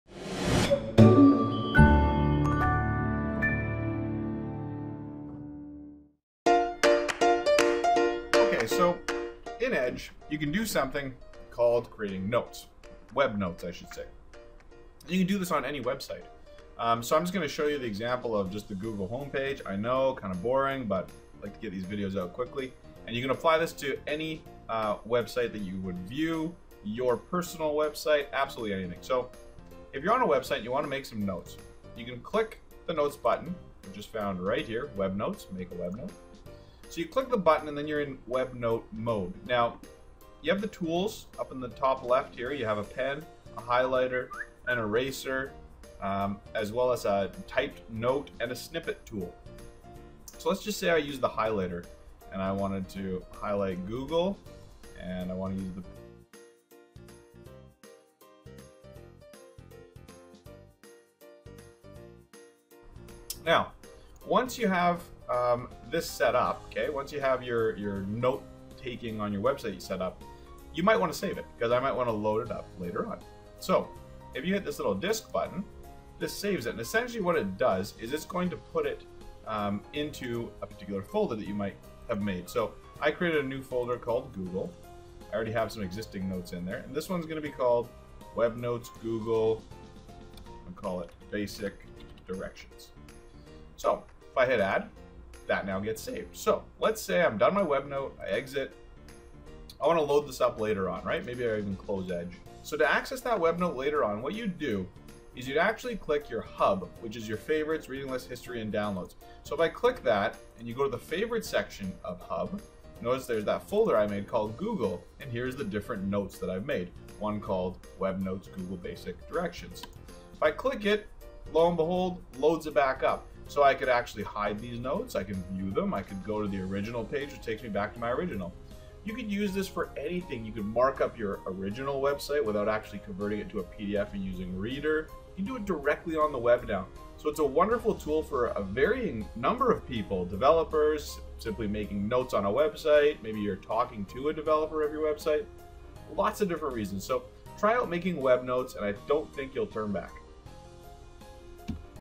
Windows 10 - Creating Basic Web Notes in Edge
In this video you will discover how simple it is to add markup and notes to a website. Learn how you can literally draw on the surface of the web.  You can even access your markup and notes later by saving your notes to favorites. Once saved you can access them through hub.